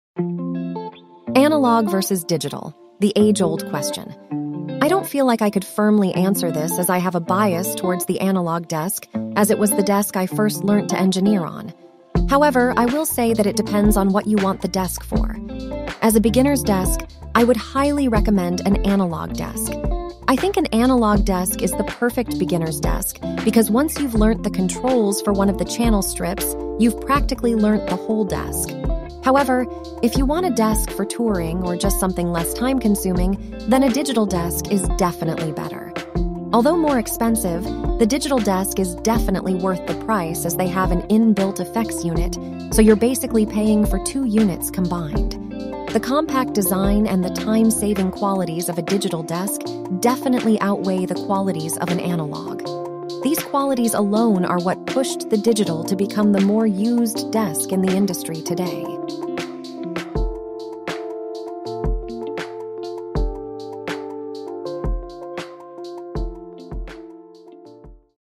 My overall opinion (Analogue vs Digital)
What do i think about these two desks and why?